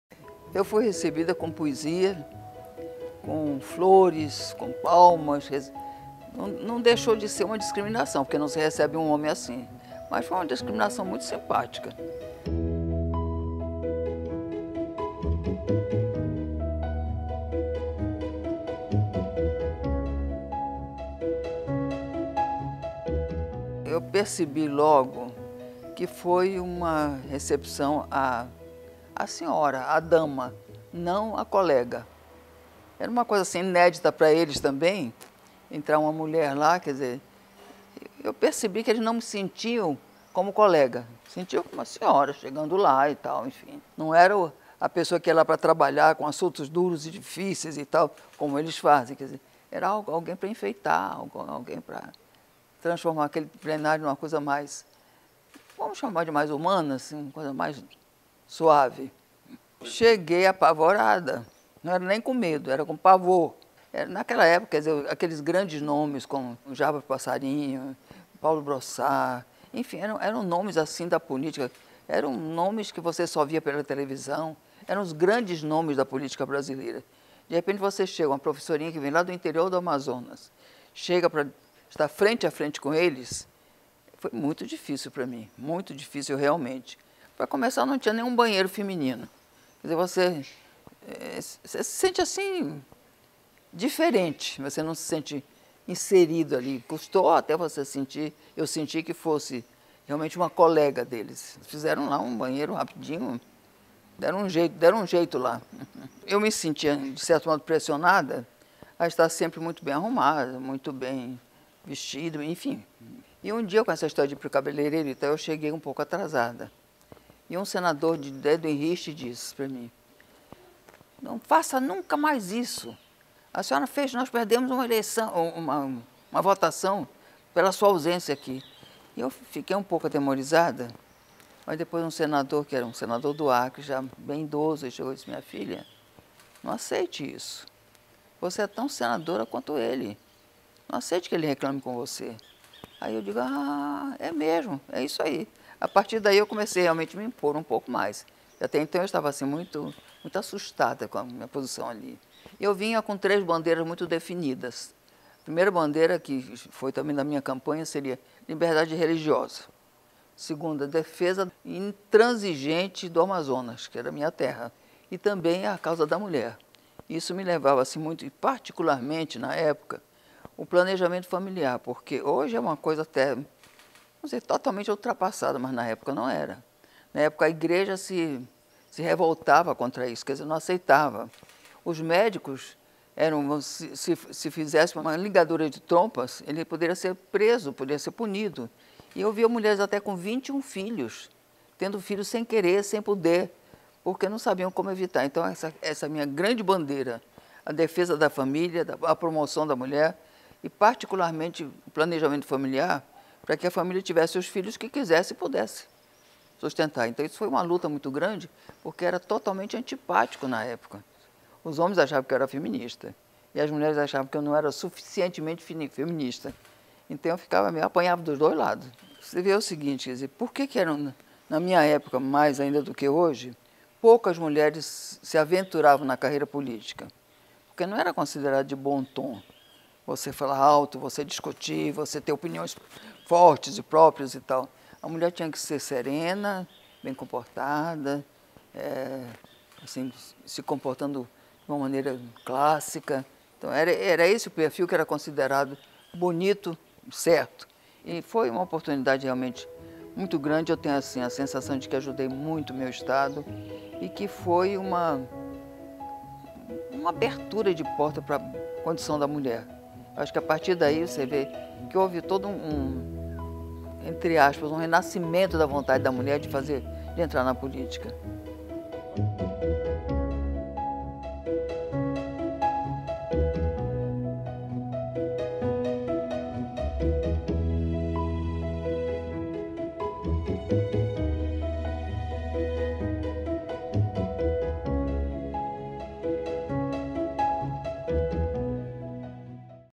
Há 40 anos, primeira senadora eleita foi recebida com flores e poesia
Eunice Michiles chegou ao Senado em 1979 e enfrentou resistência dos colegas homens, que não aprovaram os projetos de lei dela para garantir direitos às mulheres.

Este vídeo é parte do Arquivo S, publicado pela Agência Senado, sobre os 40 anos da posse da primeira senadora do país.